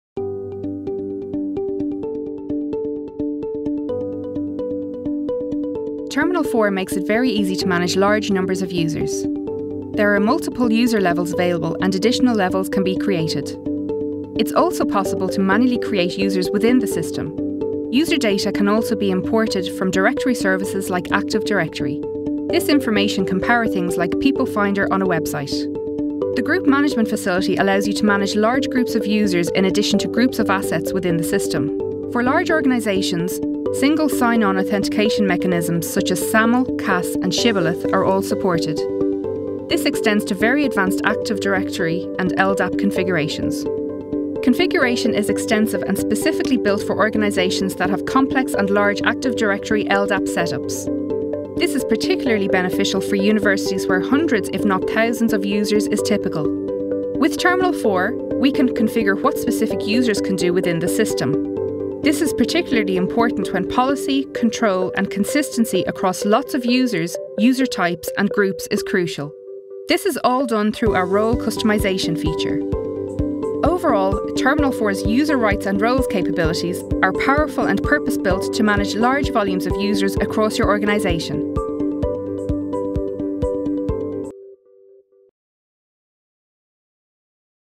User Management
Discover how TERMINALFOUR makes it very easy to manage large numbers of users. There are multiple user levels available and the group management facility allows you to manage large groups of users and teams of users.